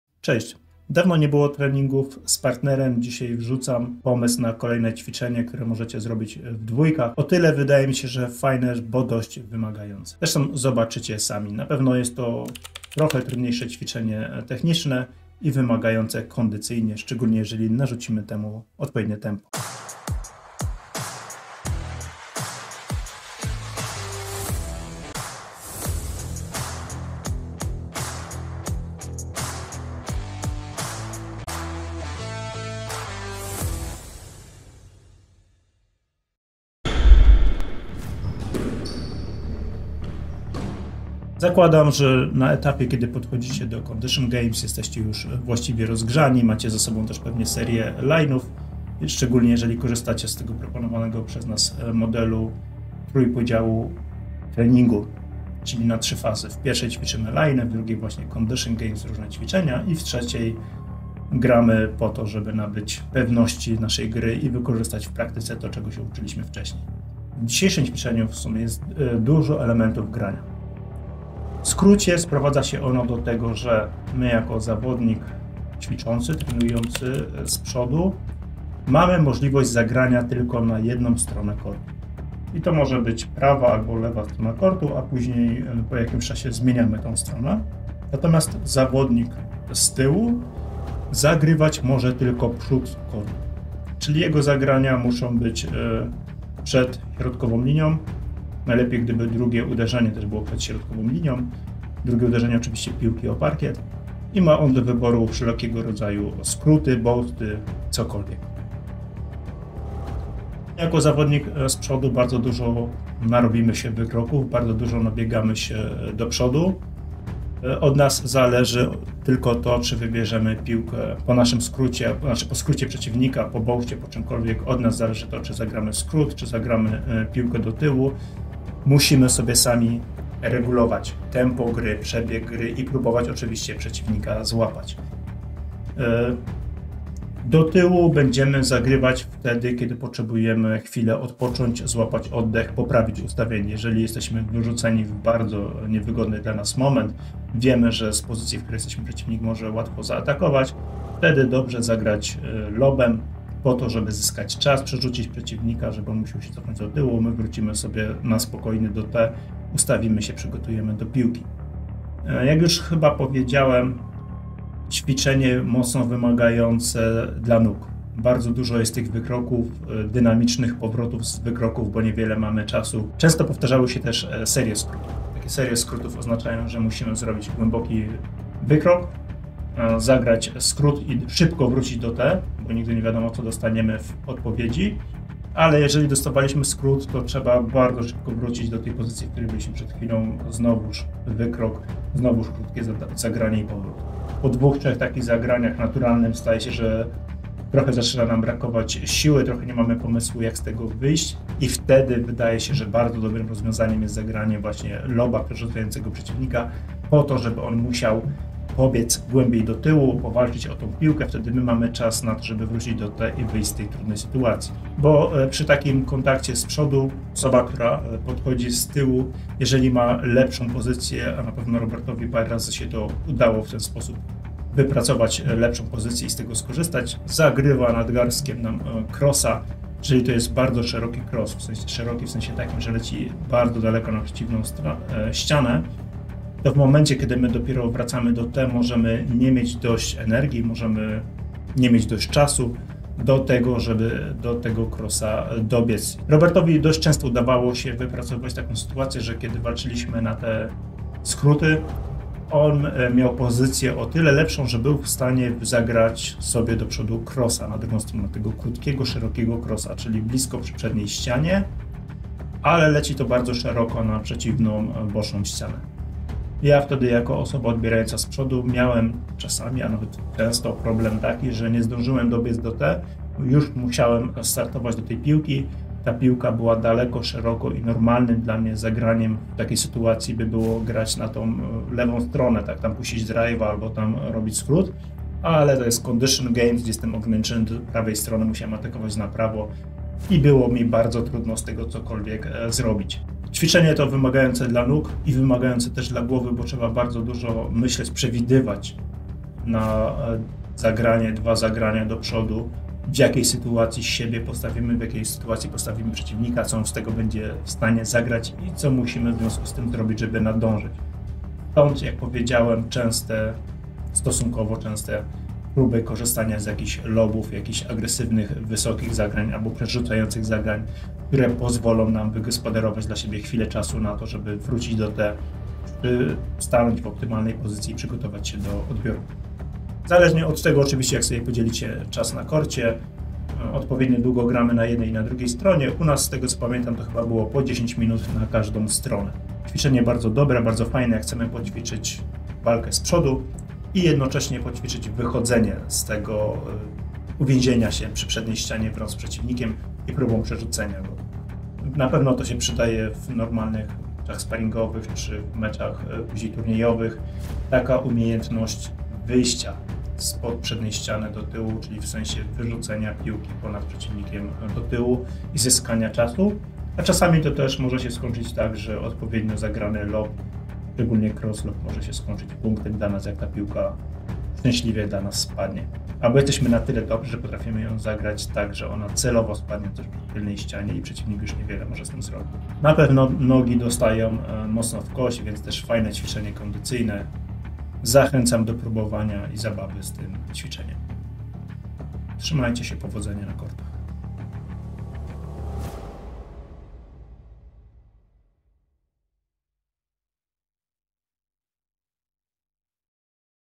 Squash pod ścianą - trening dla 2 ★★★★☆
Propozycja treningu dla 2 osób, który może być albo przyjemną odmianą, albo sposobem na sprawdzenie wytrzymałości mięśni nóg.

A jeśli ci się spodobało i chcesz abym tworzył więcej takich treści, oraz żeby były lepsze, możesz mnie wesprzeć tutaj